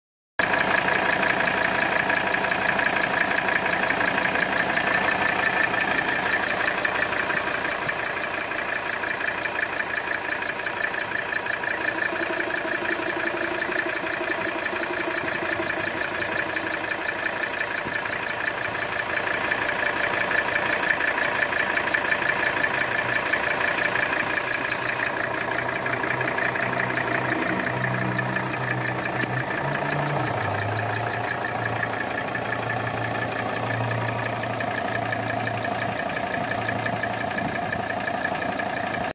1983 Mercedes-Benz 300TD Engine Running and blowby test
Just the gentle purr of a 300TD, and showing there is no blowby.